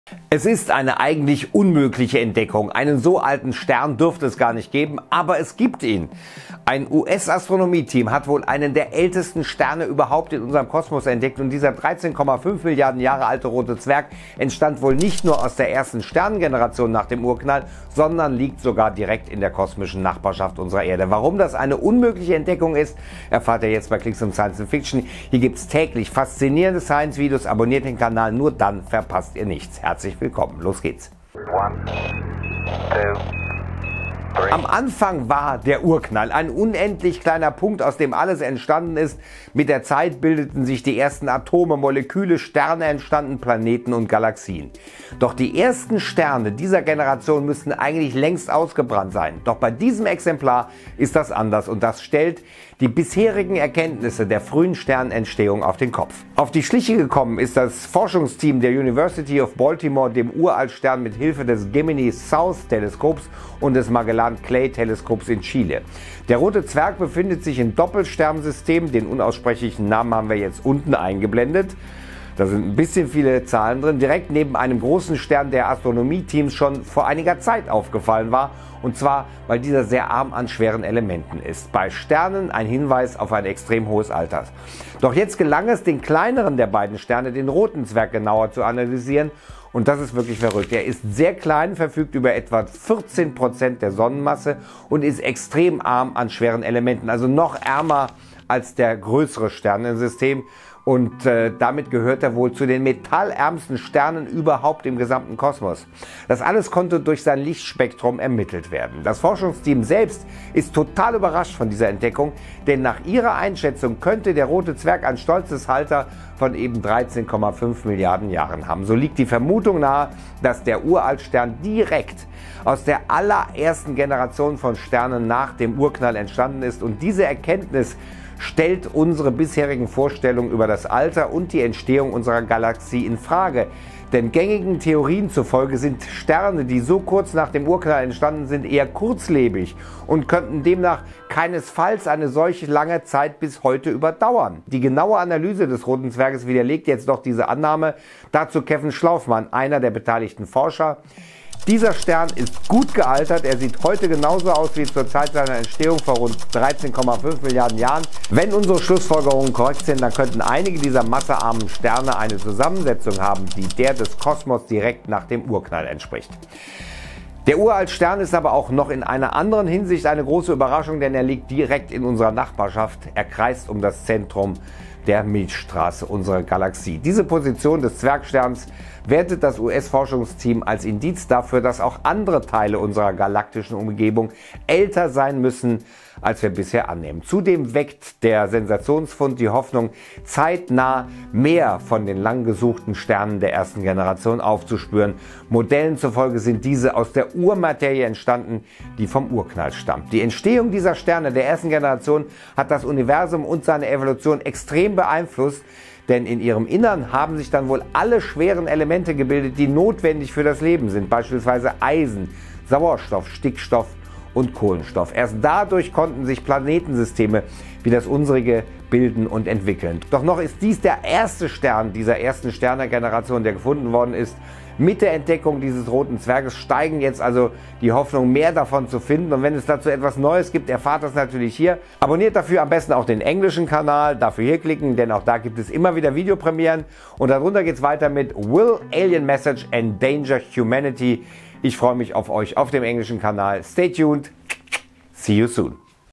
Sensationelle Entdeckung: Ältester Stern des Kosmos in direkter Nähe zur Erde!
Einen so alten Stern dürfte es gar nicht geben: Ein US-Astronomen-Team hat wohl einen der ältesten Sterne überhaupt in unserem Kosmos entdeckt. Und dieser 13,5 Milliarden Jahre alter Rote Zwerg entstand wohl nicht nur aus der ersten Sternengeneration nach dem Urknall, sondern liegt sogar direkt in der kosmischen Nachbarschaft unserer Erde und zwar im Doppelsternsystem  2MASSJ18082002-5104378.

Quelle: Thubmnail - M. Garlick/University of Warwick/ESO

Schnitt: Vincent Viebig